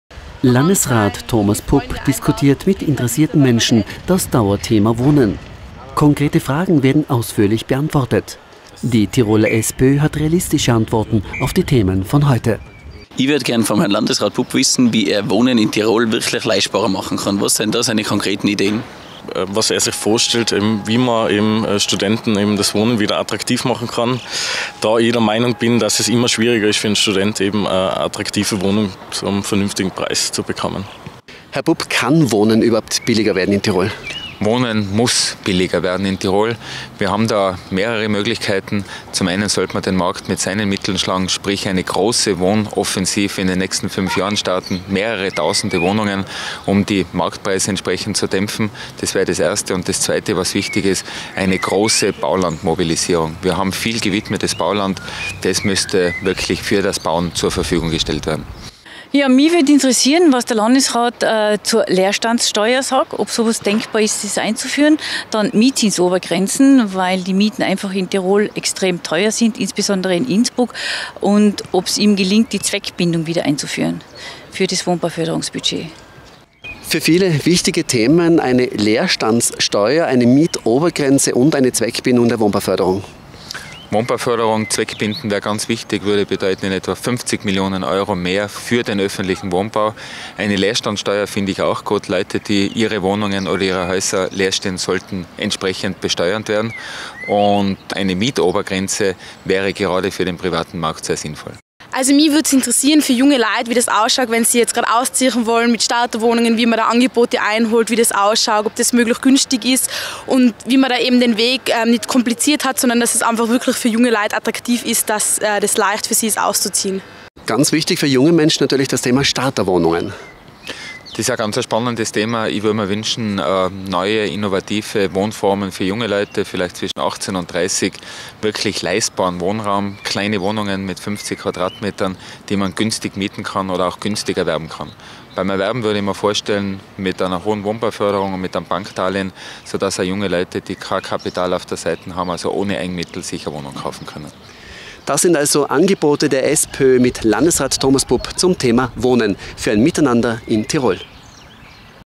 Thomas Pupp zum Thema WOHNEN
Auf tirol tv präsentiert SPÖ - Landesrat Thomas Pupp realistische Antworten auf aktuelle Themen wie Wohnen.